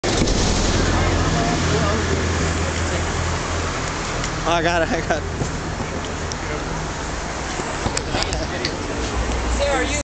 Andy Dick thinks a video camera is a photo camera
Andy Dick thinks a video camera the size of a small child is a picture camera, Can you say substance abuse.